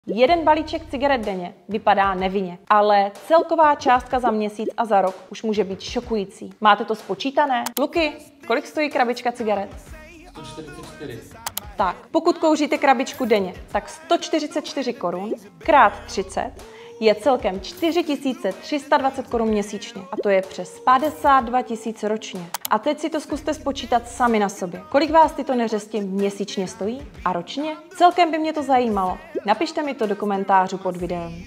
Jedna krabička cigaret denně - máte to spočítané?
Zamyslete se, co byste s těmito penězi dělali místo neřestí. Co kdybyste si je začali spořit? Za rok byste měli dost peněz na krásnou dovolenou nebo něco, co si už dlouho přejete.🏖️

Vraťte se k částce, kterou utrácíte za tyto finanční zlozvyky a zamyslete se, co byste si za tyto peníze mohli pořídit a udělalo by vám to velkou radost. Máte to?🤓

Pokud ano, právě jste našli velkou motivaci, abyste se svou neřestí něco udělali. Pokud ne, ještě chvíli přemýšlejte. Určitě je tu něco, co si toužíte splnit a brání vám v tom nedostatek peněz. Co to je? 🏠🏂🚗✨

Napište mi to do komentářů.🙋🏼‍♀️